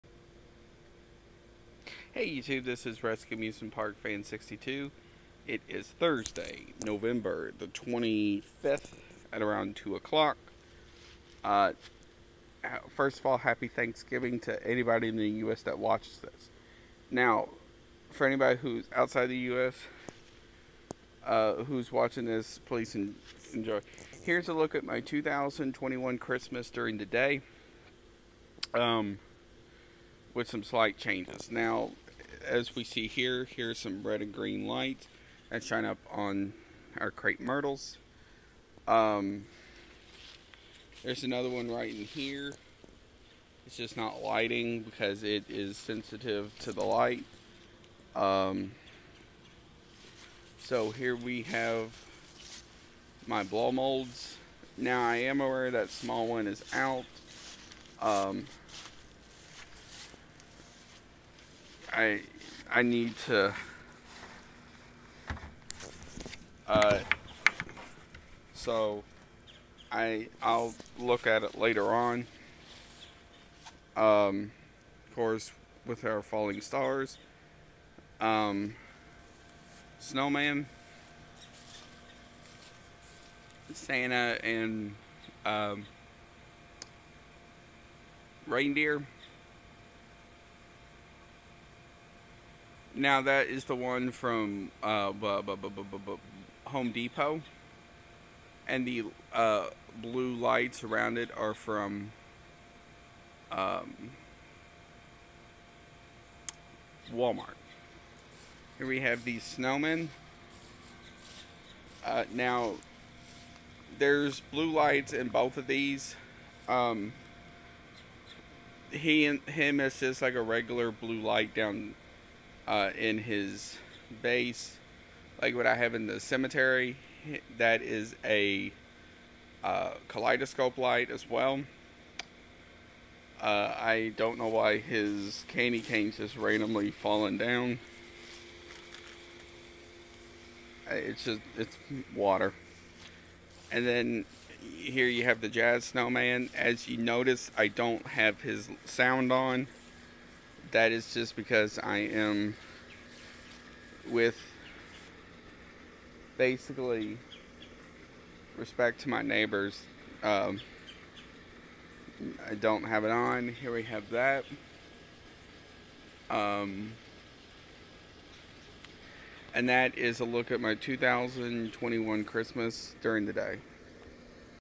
Christmas decerations 2021
Me showing my 2021 Christmas display